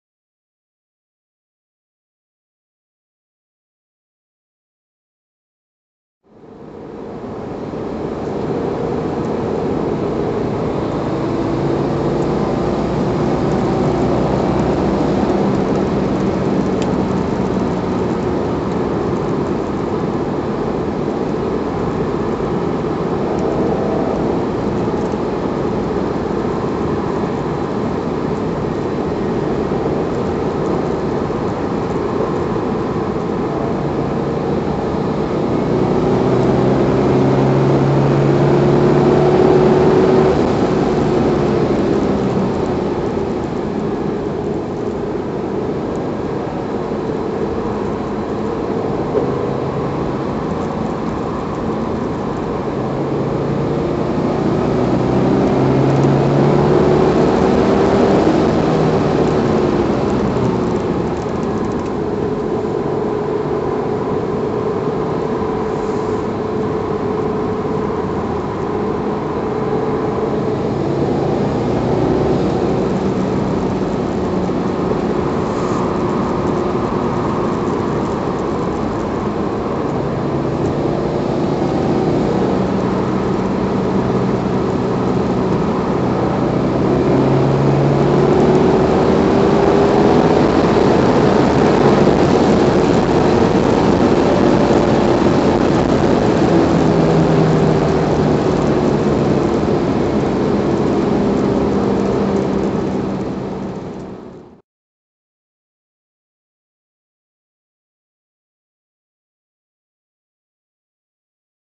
My 2009 subaru WRX 4 door somewhere near Gallatin Tennessee
First drive on some cool road I just found in Tennessee